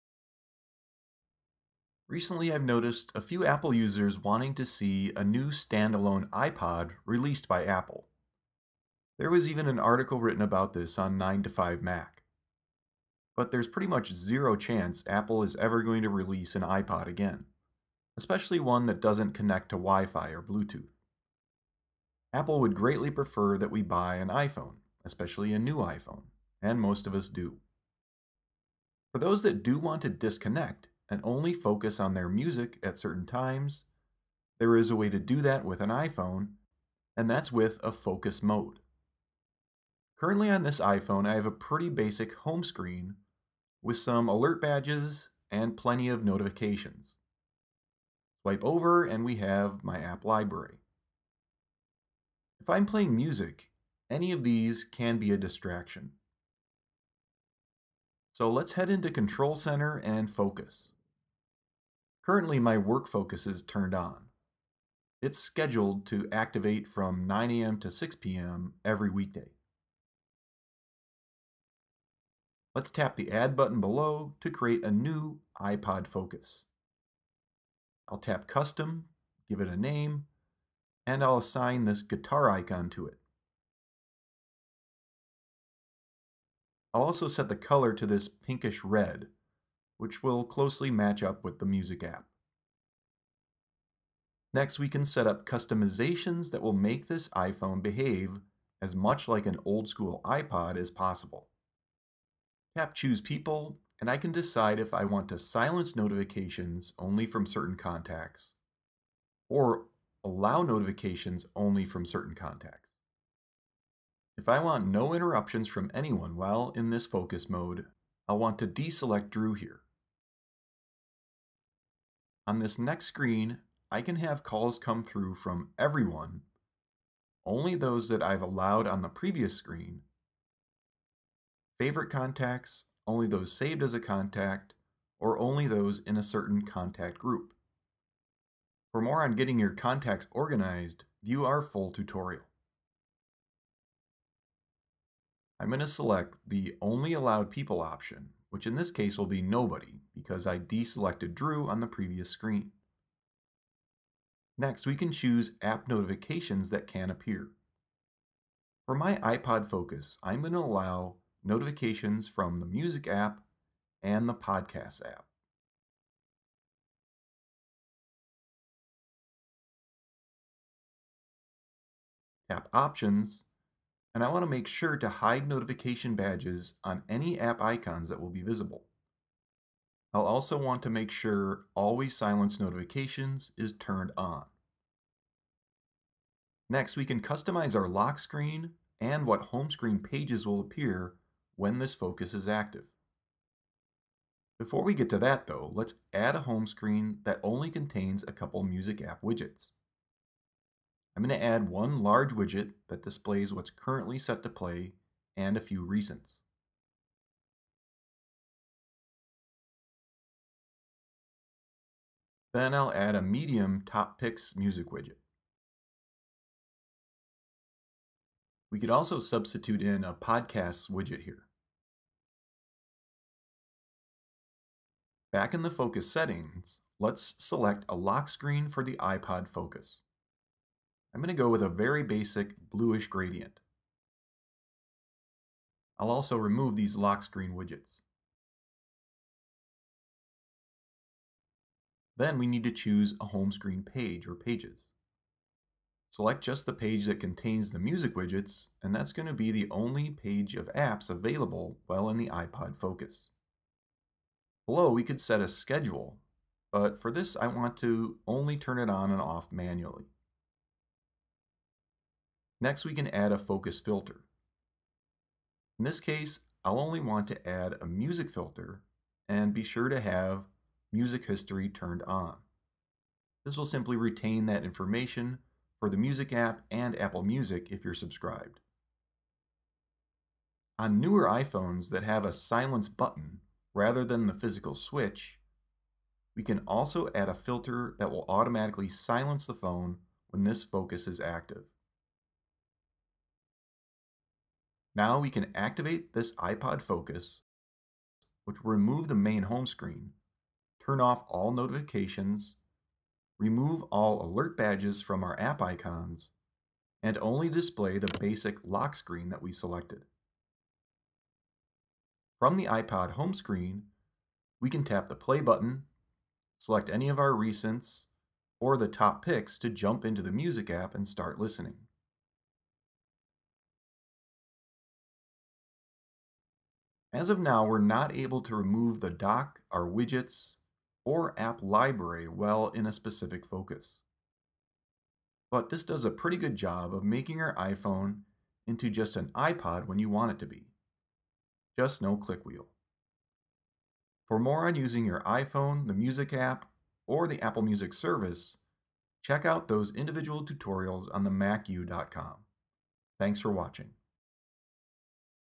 Create an iPod focus Mode to listen to music without distractions on your iPhone!
Sometimes its nice for a device to only do one thing, such as an old iPod Classic that was just a great music player. An iPhone can do a ton of things and create even more distractions! In this lesson see how to create an iPod Focus mode that removes as many distractions as possible while filling your iPhone home screen with only the music app. It's not exactly like using an old click wheel iPad but does provide a great way to focus only on your music!